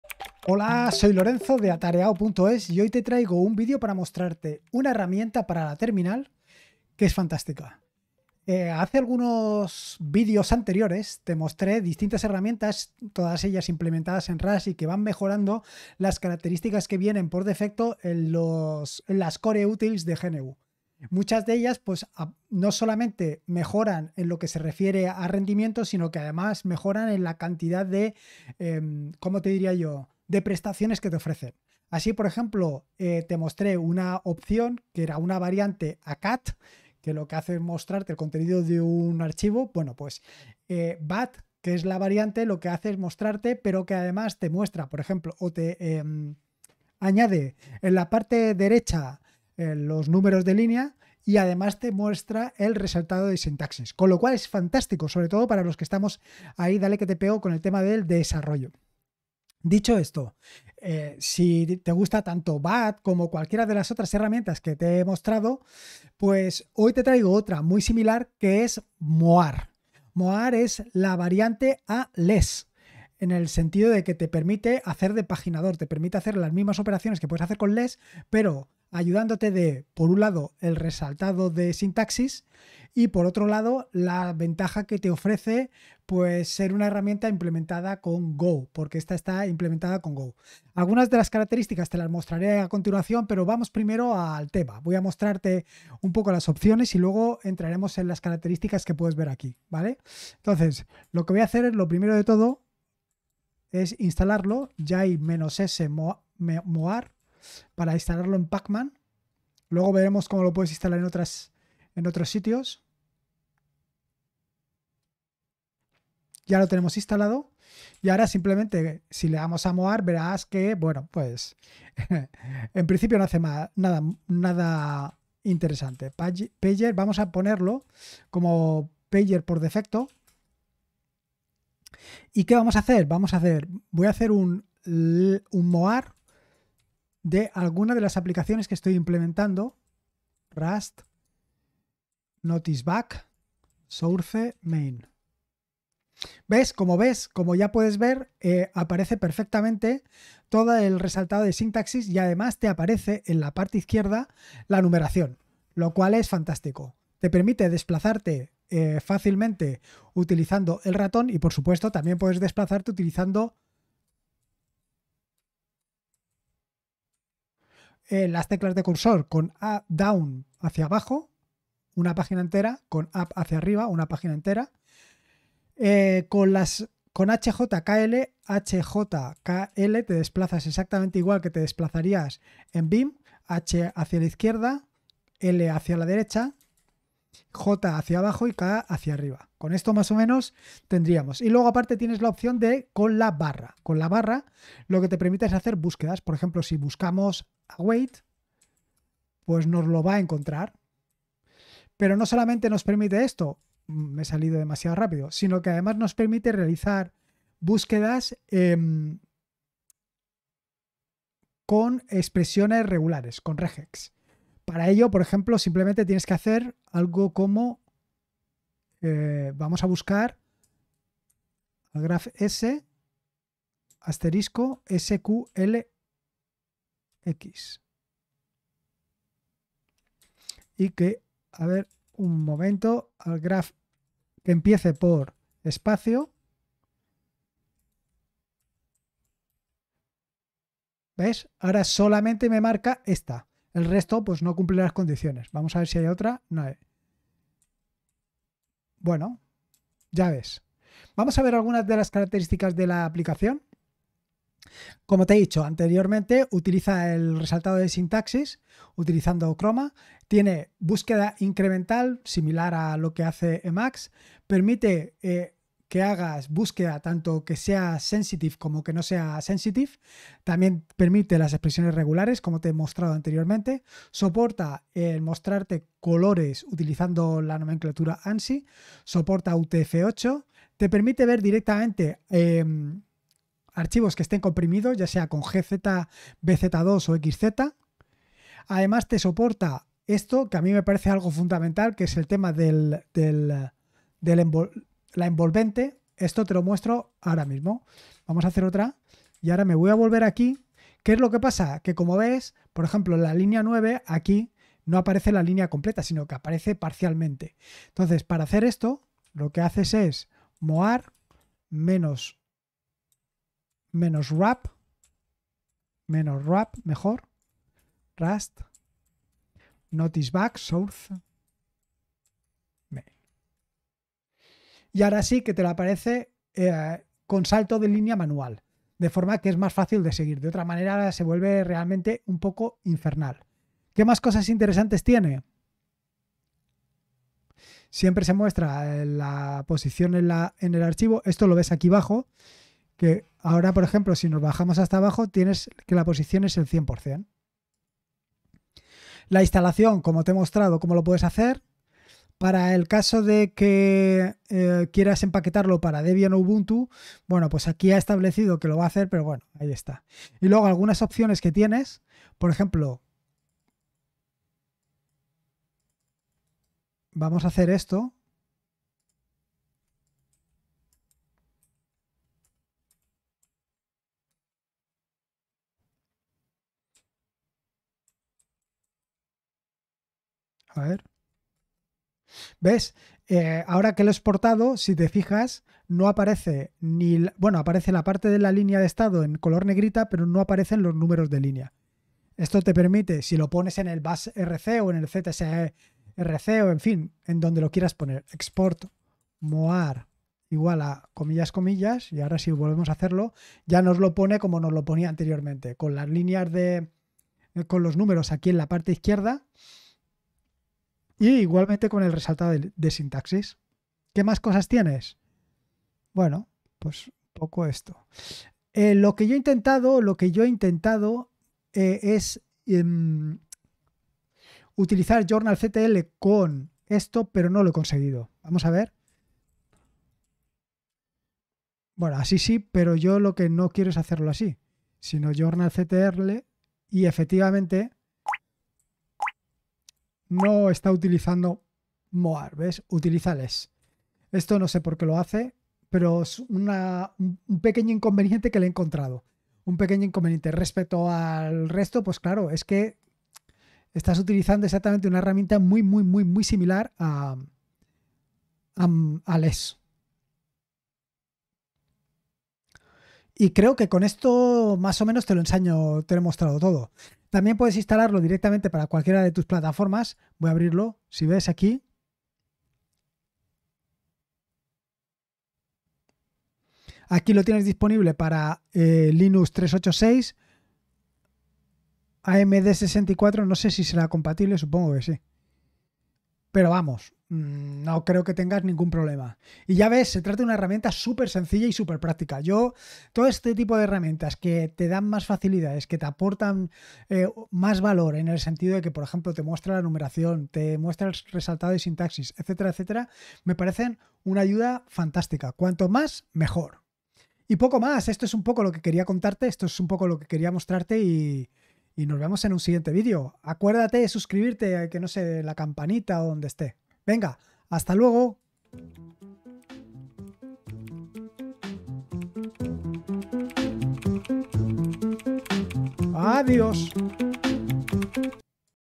Una alternativa a less con mas posibilidades. Terminal Linux
moar es una herramienta alternativa a less con resaltado de sintaxis, búsqueda mediante expresiones regulares y mas características.